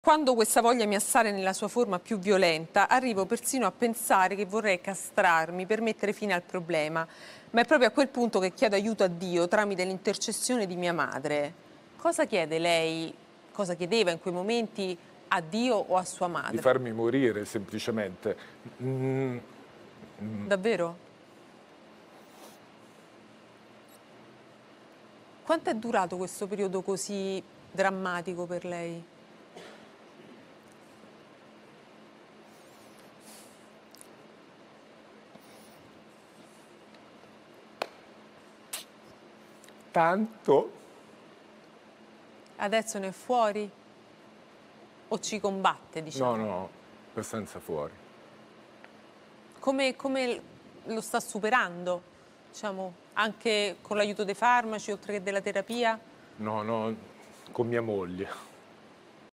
Rocco Siffredi: "Ho pensato di castrarmi”
In questa intervista, Rocco Siffredi rivela che in passato ha pensato addirittura di castrarsi per porre fine alla sua dipendenza dal sesso. L'attore racconta la sua lotta contro questo problema e come è riuscito a superarlo.